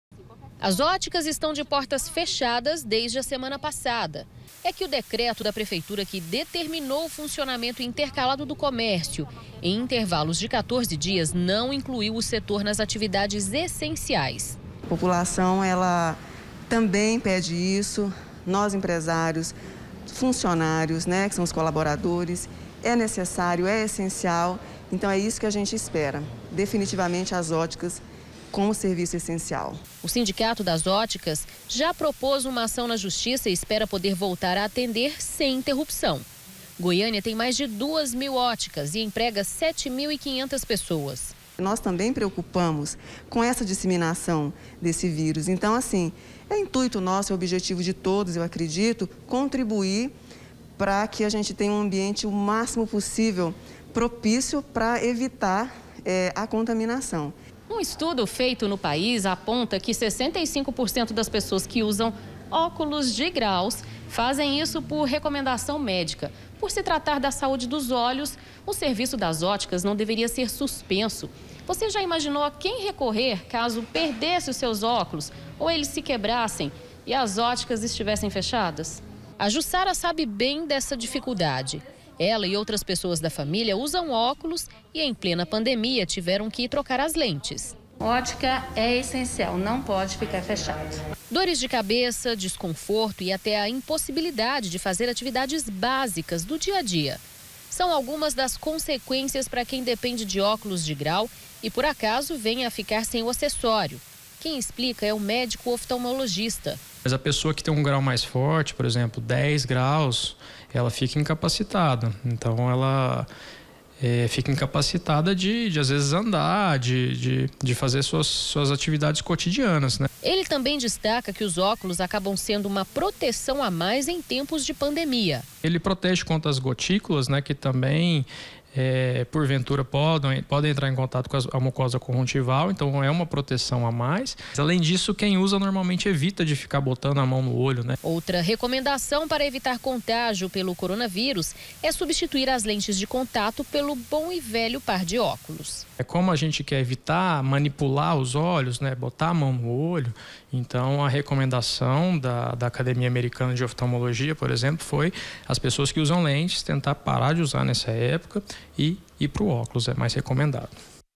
ESCALONAMENTO DO COMÉRCIO: ÓTICAS ENTRAM NA JUSTIÇA PARA REABRIR
AS ÓTICAS NÃO FORAM CLASSIFICADAS COMO SERVIÇOS ESSENCIAIS E POR ISSO TEM QUE SEGUIR O DECRETO DE ESCALONAMENTO DA PREFEITURA DE GOIÂNIA./ SÓ QUE OS ÓCULOS SÃO ESSENCIAIS PRA QUEM PRECISA E POR ISSO, EMPRESÁRIOS ENTRARAM COM AÇÃO NA JUSTIÇA PARA QUE AS ÓTICAS POSSAM  FUNCIONAR SEM INTERRUPÇÕES.